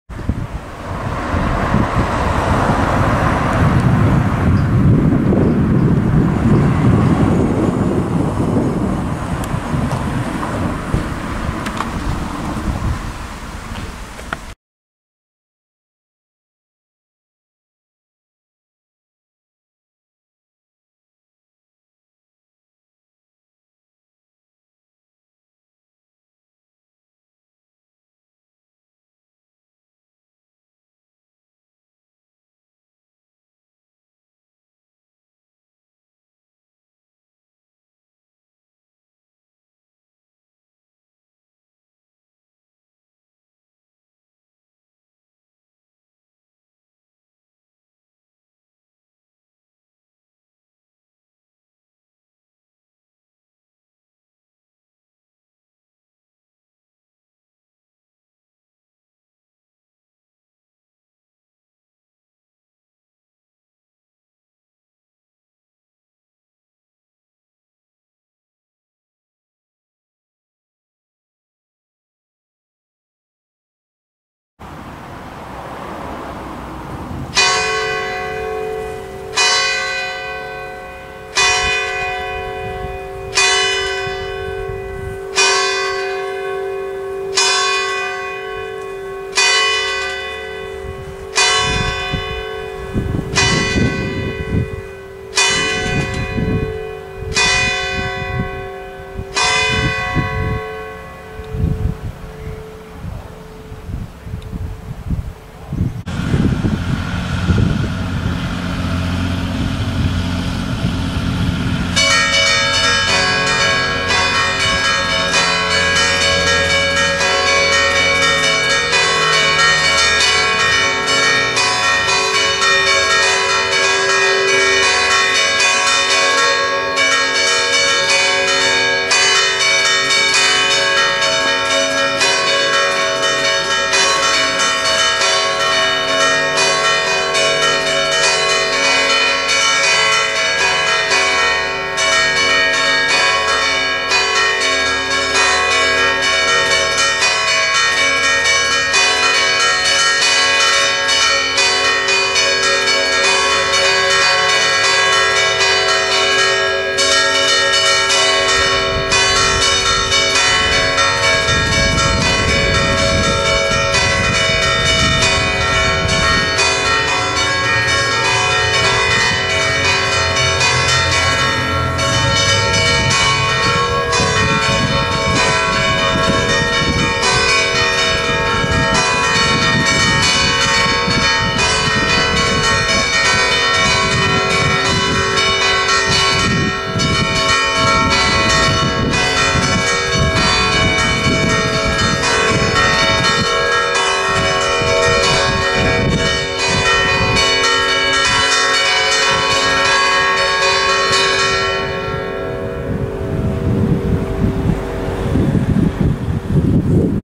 Le campane di Usseglio (TO) - Chiesa Parrocchiale dell'Assunzione di Maria - Angelus Feriale
Usseglio (TO)
Chiesa Parrocchiale dell'Assunzione di Maria, 3 campane in Fa#3 (Fa#3 - La3  - Do#4)
Suonata a Carillon per l'Angelus Feriale delle ore 12
A presto e buona visione da Campanaro07!